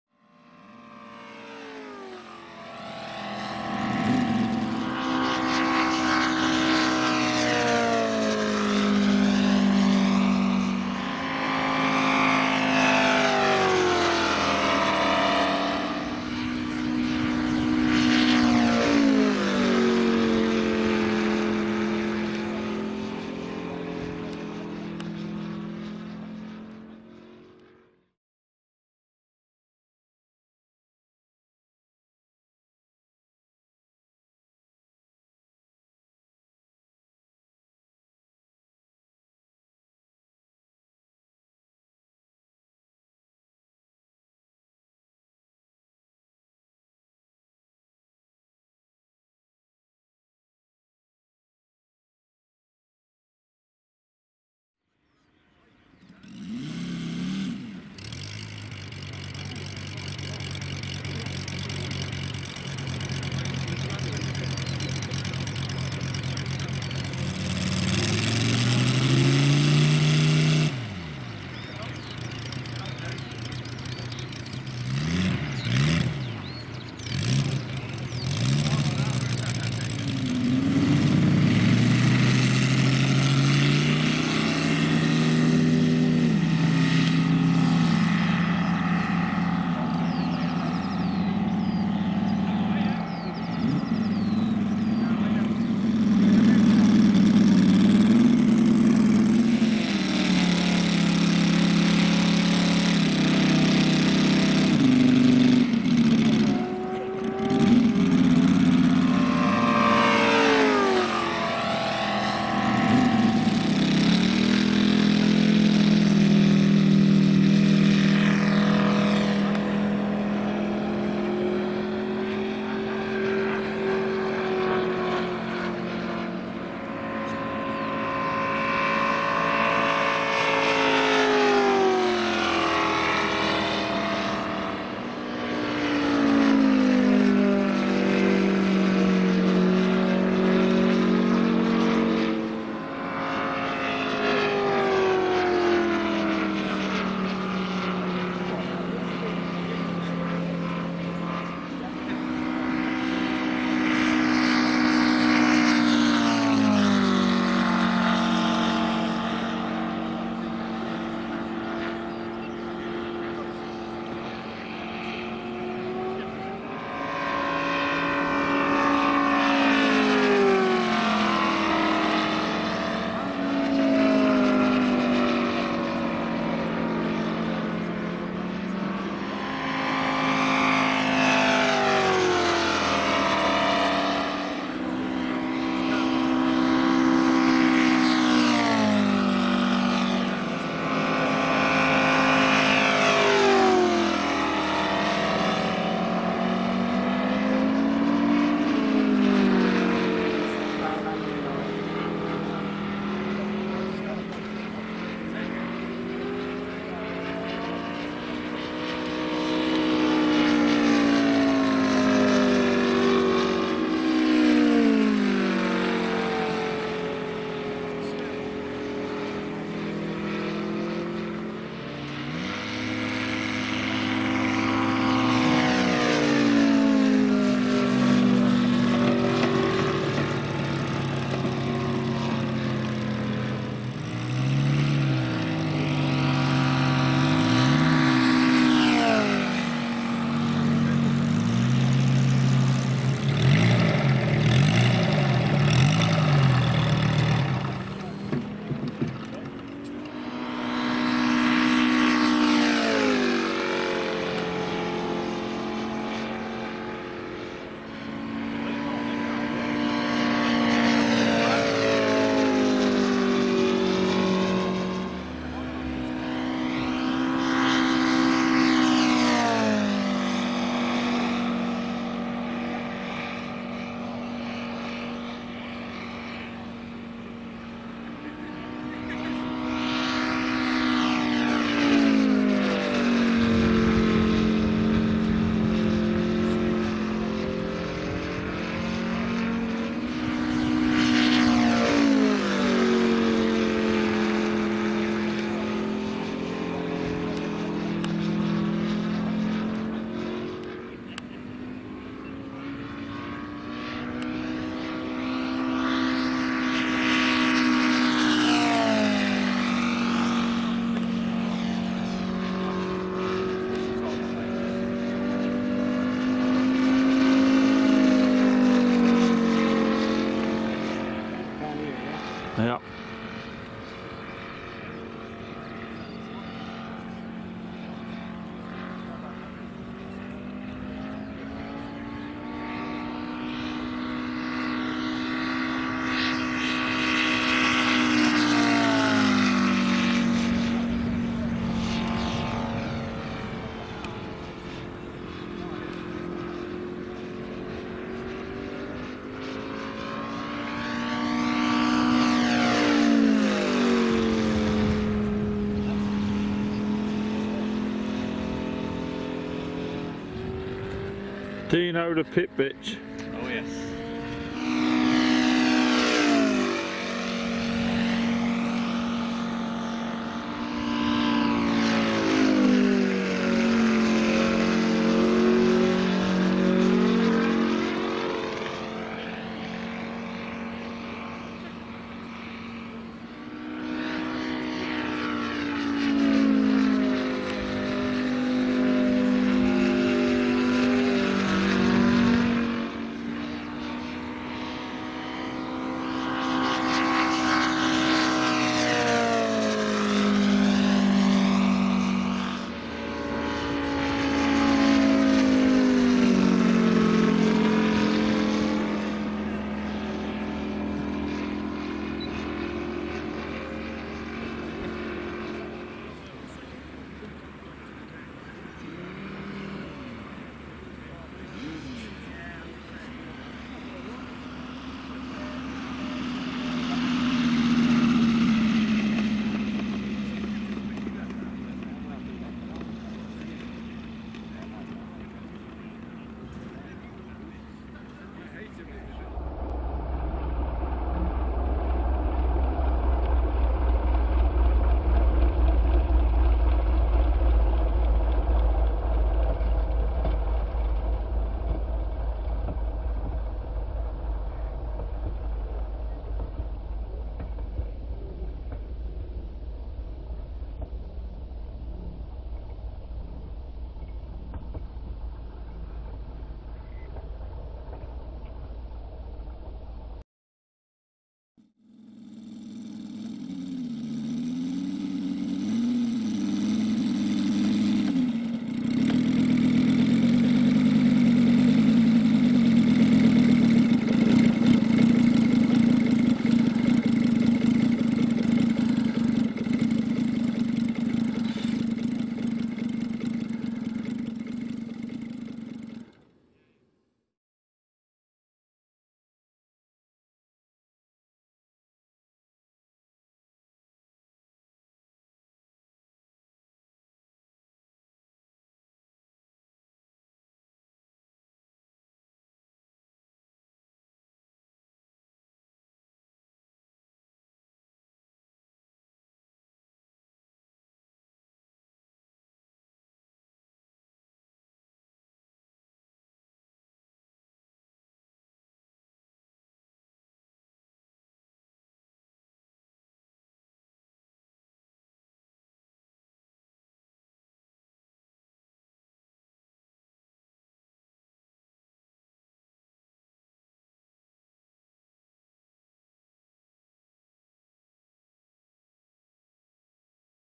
AWESOME MOKI SOUND - TWO GIANT SCALE RC P-47 THUNDERBOLTS - TREVOR & NIGEL - 2018
FILMED AT NLMFC ON 3-6-2018,